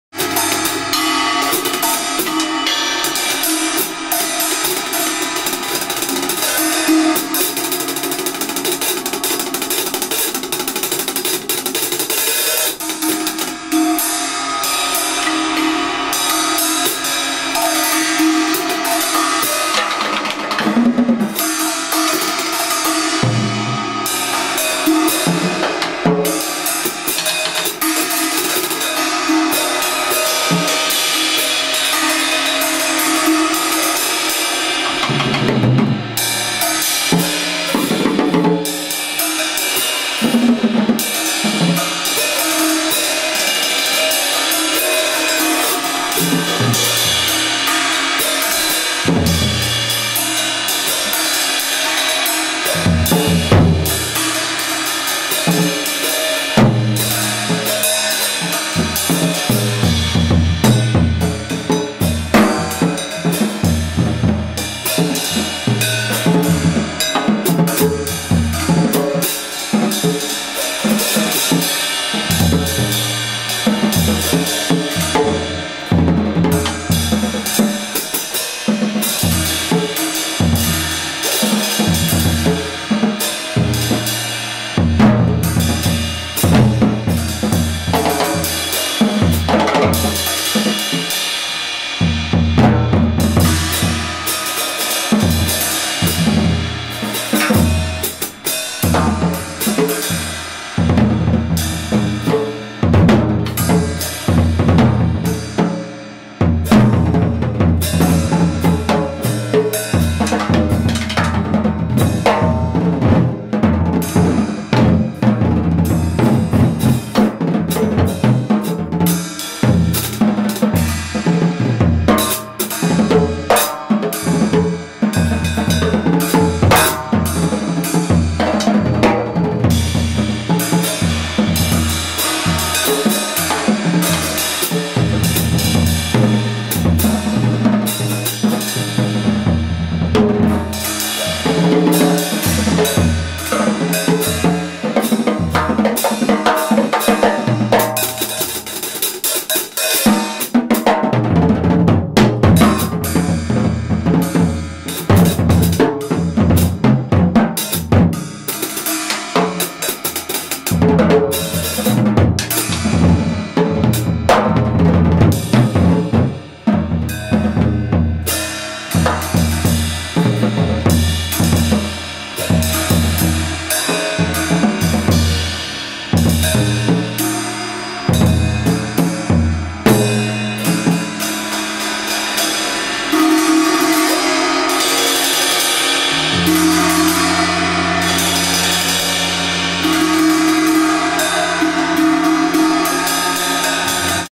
Shoji Hano drums solo @ Inage Candy
Live at Inage Candy, Chiba (Japan) / 1st May 2010 /